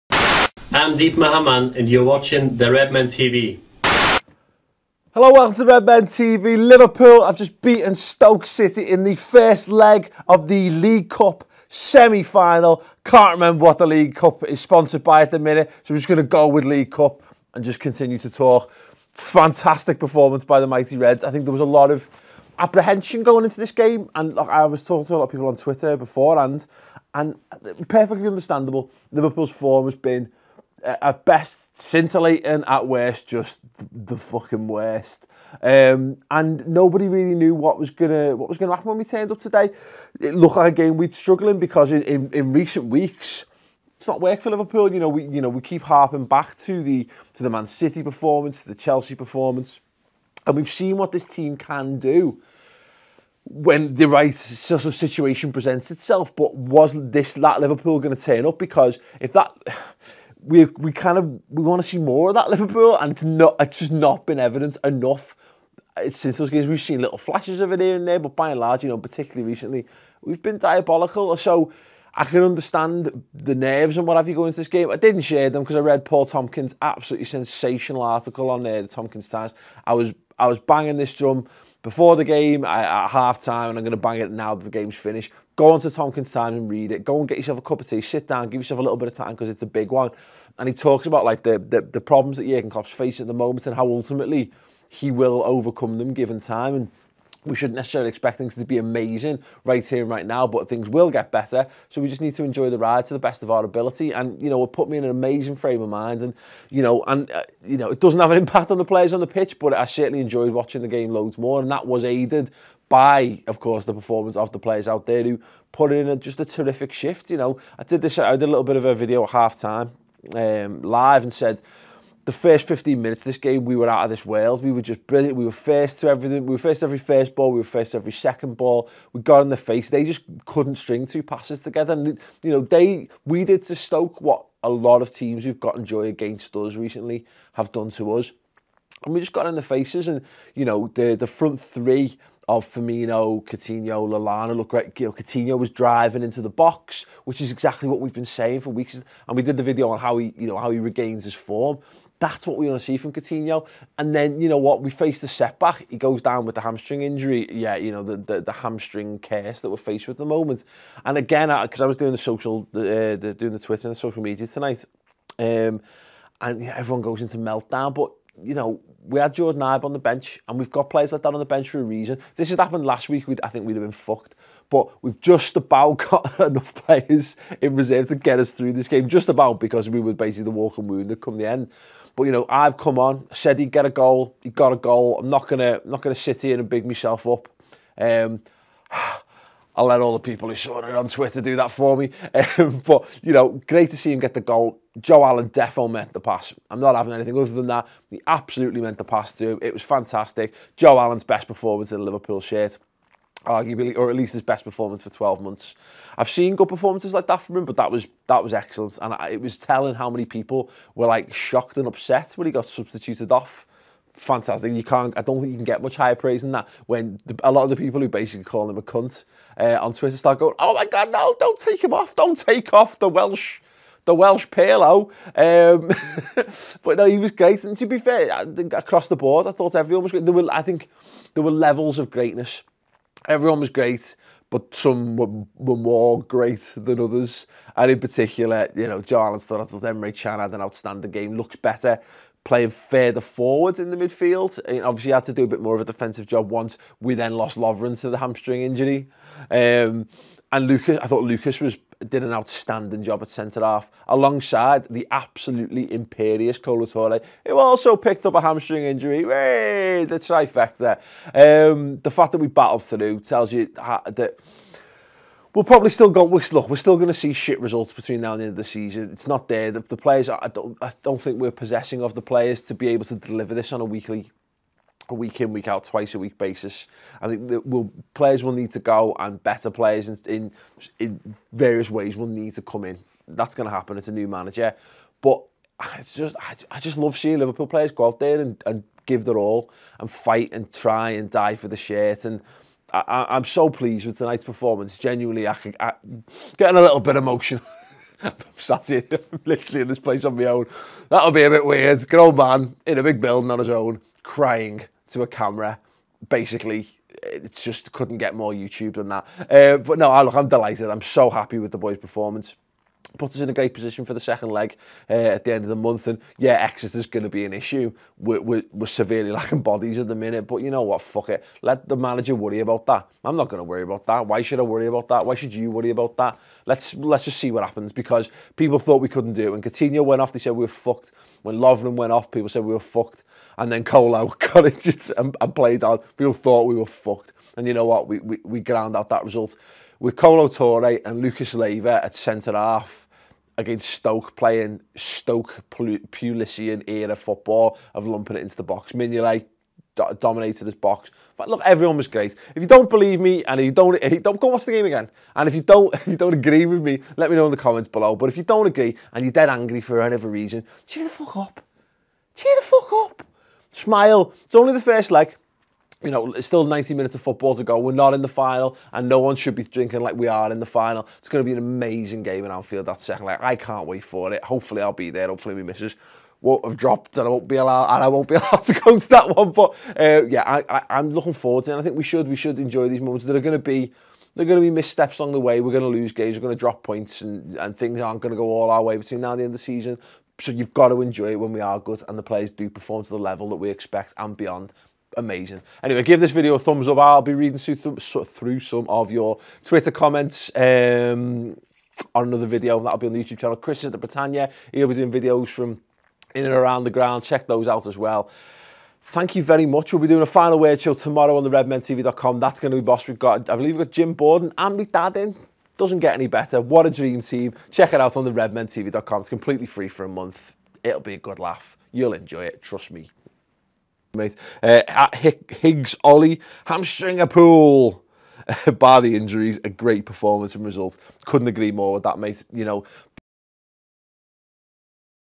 Stoke City 0-1 Liverpool: Hamstring Hattrick Can't Stop Ibe Inspired Reds! (Match Reaction)
Liverpool will take a 0-1 lead into the Capital One Cup Semi 2nd leg after a Jordon Ibe goal saw them deservedly win against a dogged Stoke City side. Despite losing Coutinho, Lovren and more or less Toure to hamstring injuries the makeshift reds did enough to win the game...